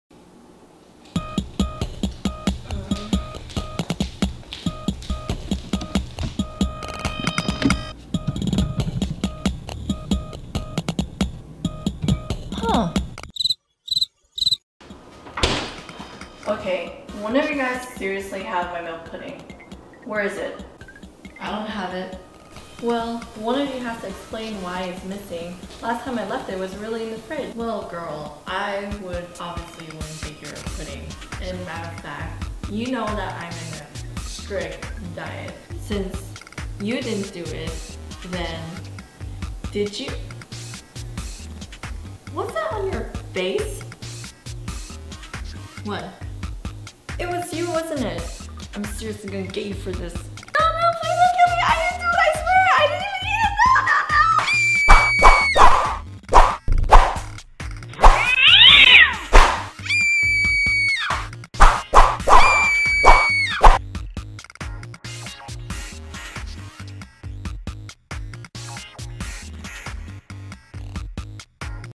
The Missing Pudding: Aftereffects Final video project
My pudding is missing!

Who could've stole it?